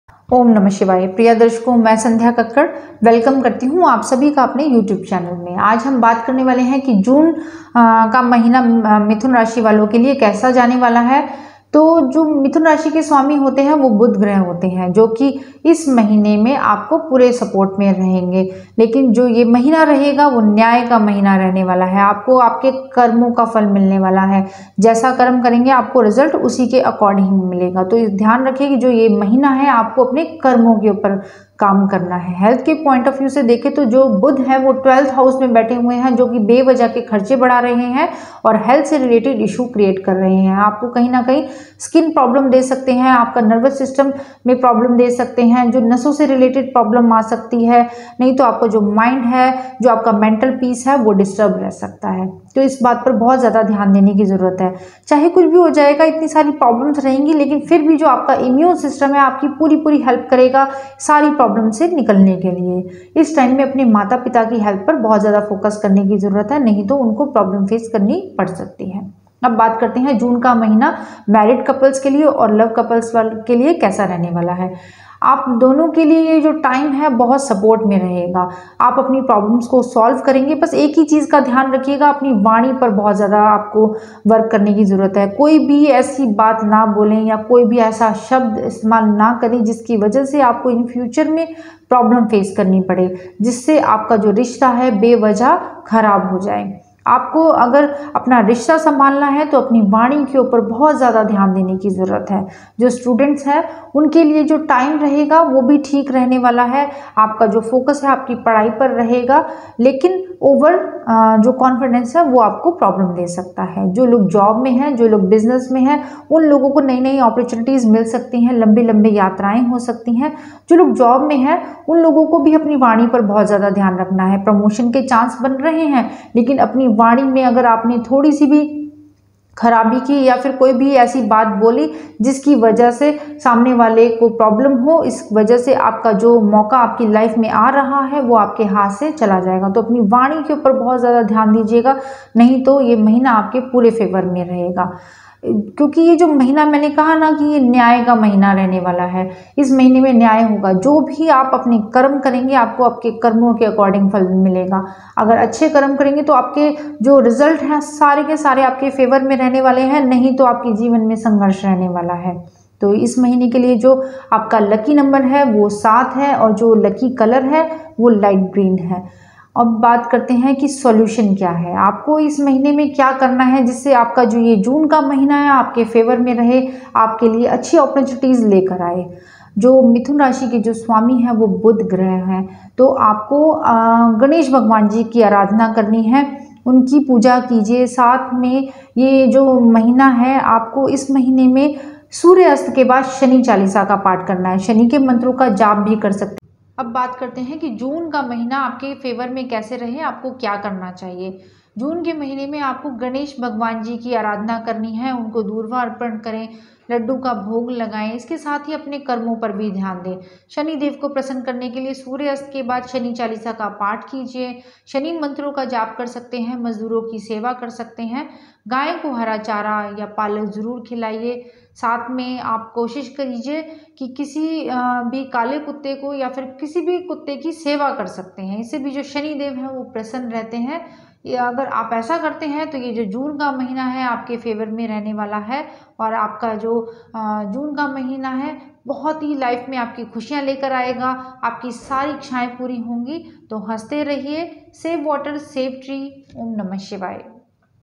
Mithun Rashi | Gemini predictions for June 2022 Rashi Fal | Monthly horoscope | By Sandhya Kakkar
Mithun Rashi | Mithun predictions for June 2022 Rashi Fal | Monthly horoscope | Sandhya Kakkar

Iss video Mei maine apko Mithun Rashi valo ke liye June ka mahina kya result lekr aane vala hai or kin kin baato ka apko dhyn rkhna chhaiye or kya upaye kre jisey June ka mahina aap ke favour Mei aa sake .

NOTE :
Jo bhi rashifal iss video Mei maine apko btaya hai vo apki kundali ke hisab se jo bhi apki Rashi hai yani ki chandra rashi ke hisab se hi yeh rashifal dia gya hai .